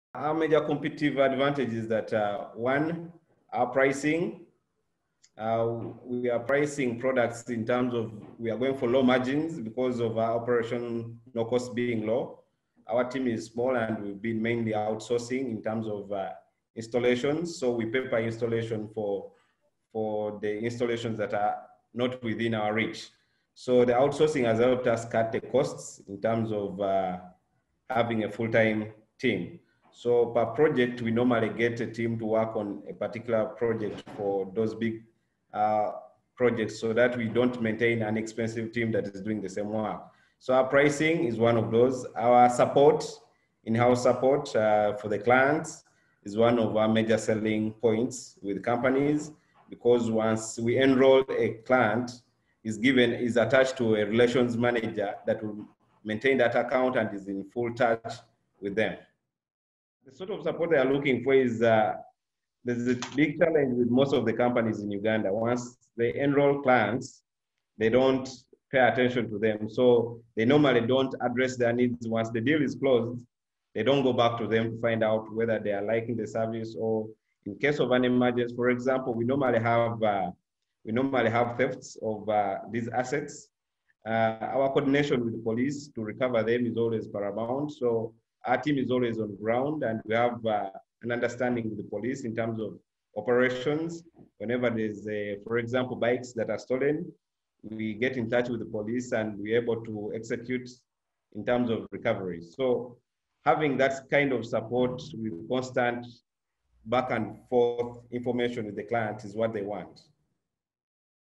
Tracking and Fleet Management in Uganda: Competitive Advantages of Track24 by Mas Yunus
Masaba 'Mas' Yunus, Founder and CEO of Track24, a leading provider of fleet management solutions, explains what are the competitive advantages of the company.